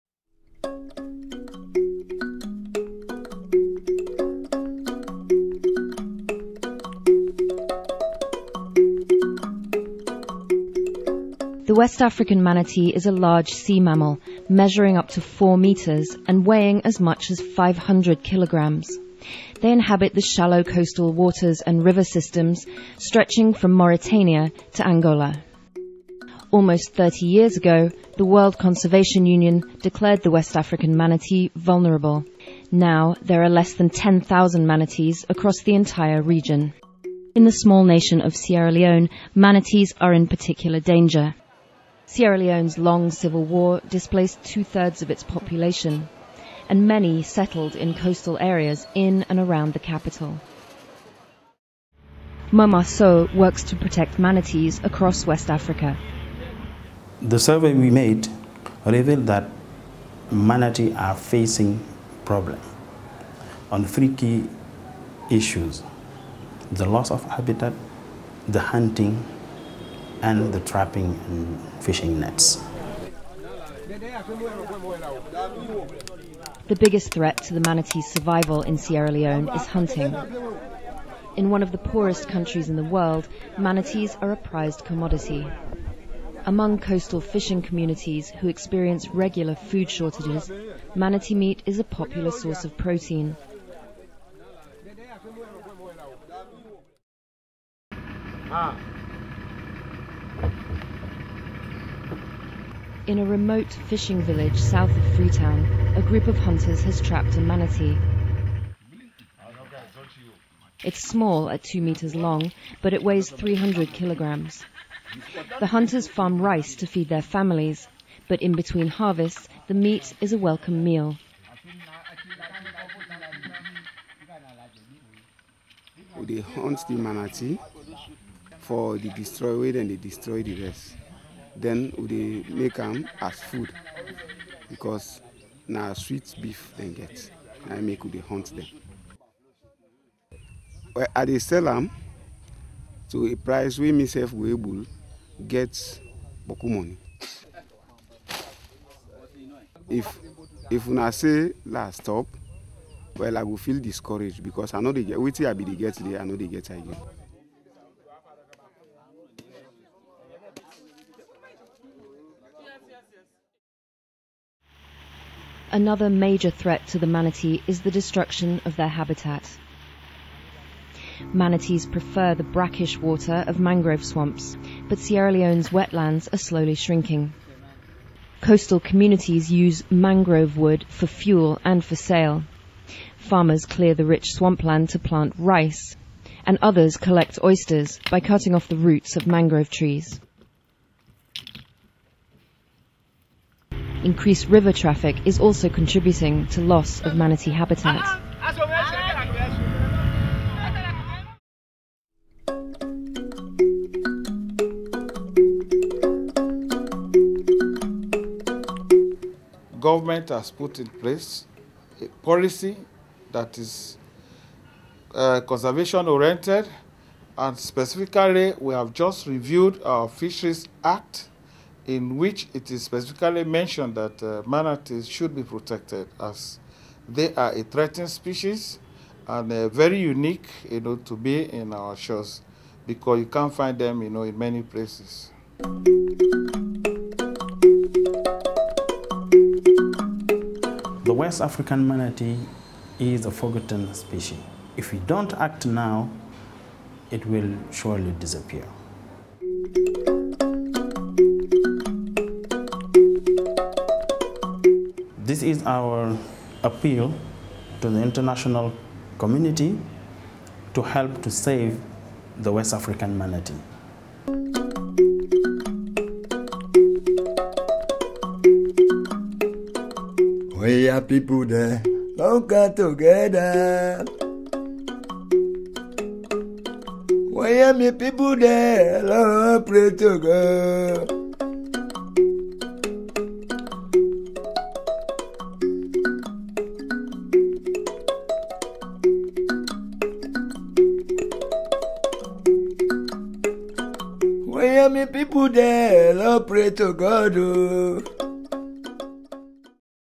Why West Africa's Manatee deserves top CMS and CITES protection
The African Manatee is a member of the Trichechidae; a sirenian, mermaid-like sea cow that can grow up to 3 meters and weigh 500 kilograms.

Wetlands International is calling for tougher protection measures to ensure the survival of this endangered species.